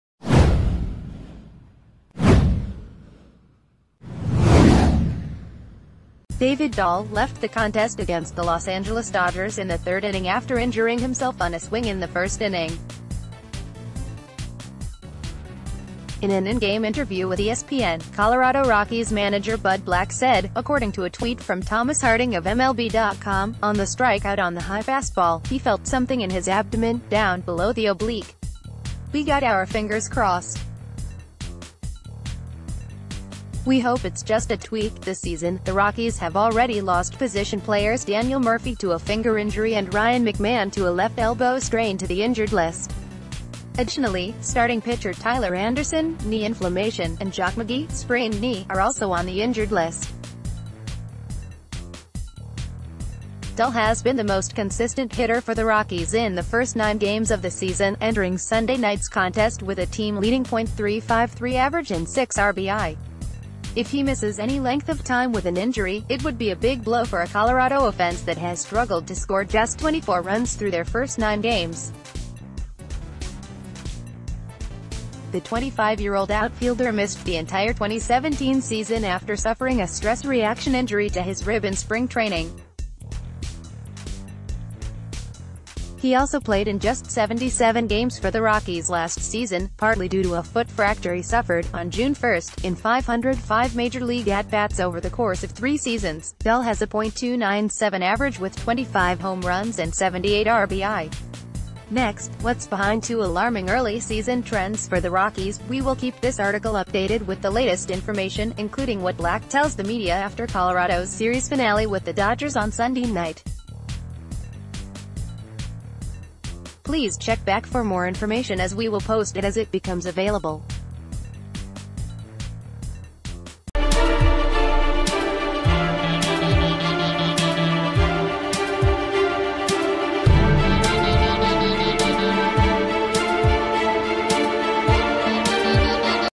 Colorado Rockies injury news: Bud Black discusses David Dahl’s departure
David Dahl left the contest against the Los Angeles Dodgers in the third inning after injuring himself on a swing in the first inning. In an in-game interview with ESPN, Colorado Rockies manager Bud Black said, according to a tweet from Thomas Harding of MLB.com, “On the strikeout on the high fastball, he felt something in his abdomen, down below the oblique. We got our fingers crossed. We hope it’s just a tweak.”This season, the Rockies have already lost position players Daniel Murphy to a fing...